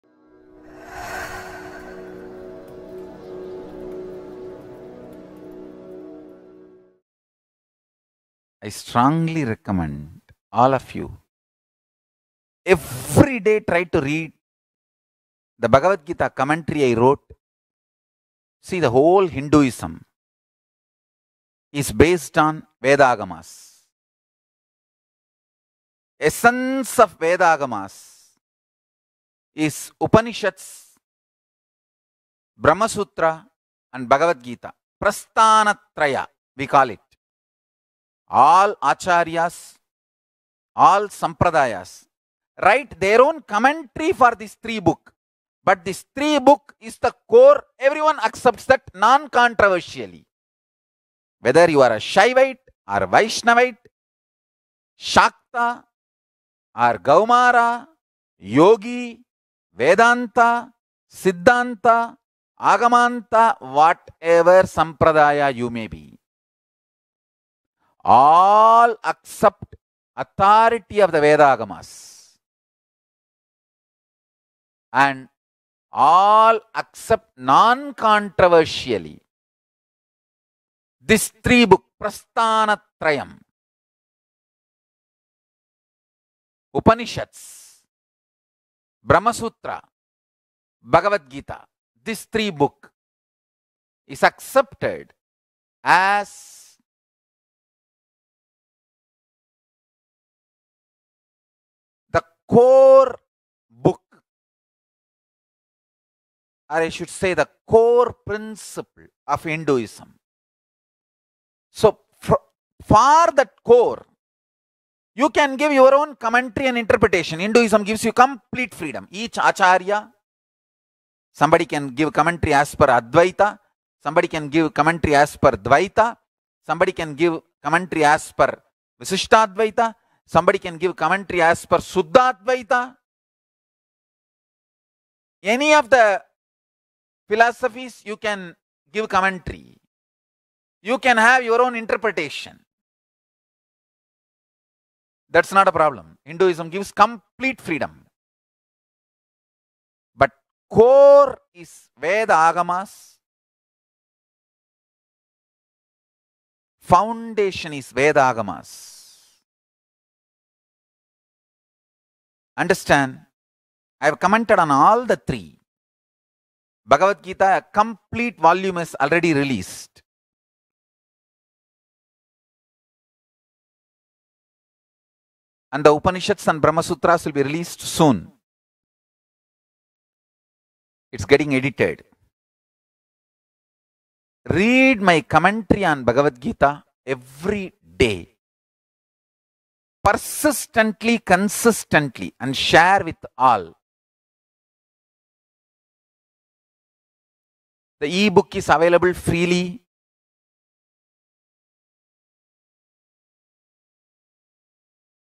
Essence of Vedas are These 3 : Upanishads, Brahma Sutras & Bhagavad Gita
I strongly recommend all of you, every day try to read the Bhagavad commentary I wrote, see the whole Hinduism is based on Veda-Agama-s. Essence of Veda-Agama-s is Upanishad-s, Brahma Sutra and Bhagavad Gita, ‘Prasthanatraya’ we call it, all acharya-s, all sampradaya-s write their own commentary for these 3 book, but these 3 book is the core, everyone accepts that non controversially, whether you are a Shaïvite or Vaishnavite, Shakta or Gowmara, yogi, Vedanta, Siddhanta, Agamanta, whatever sampradaya you may be, all accept authority of the Veda-Agama-s and all accept non controversially these 3 book ‘ Prasthanatrayam’ Upanishad-s, Brahma Sutra, Bhagavad Gita, these 3 book is accepted as the core book or I should say the core principle of Hinduism, so for that core you can give your own commentary and interpretation, Hinduism gives you complete freedom. Each acharya, somebody can give commentary as per Advaïta, somebody can give commentary as per Dvaïta, somebody can give commentary as per Vishishta Advaita, somebody can give commentary as per Shuddhadvaita, any of the philosophies you can give commentary, you can have your own interpretation that is not a problem, Hinduism gives complete freedom but core is Veda-Agama-s, foundation is Veda-Agama-s. Understand I have commented on all the 3, Bhagavad Gita, a complete volume is already released and the Upanishad-s and Brahma Sutra-s will be released soon, it’s getting edited. Read my commentary on Bhagavad Gita every day, persistently, consistently and share with all, the e-book is available freely.